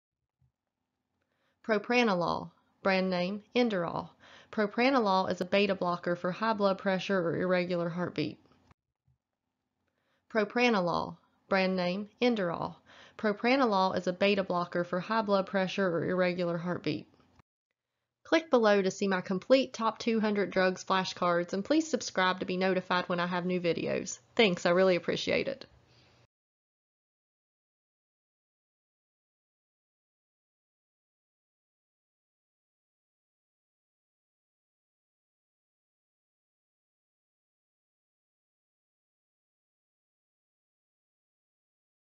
Propranolol Pronunciation - Generic Name, Brand Name, Indication (Top 200 Drugs) PTCB NCLEX NAPLEX
Propranolol (Inderal) Pronunciation - Generic Name, Brand Name, Indication, Drug Class (Top 200 Drugs). Individual Video Flashcards with Audio of the Top 200 Drugs for pharmacy students, pharmacy technicians, and nursing students preparing for NAPLEX, PTCB, or NCLEX.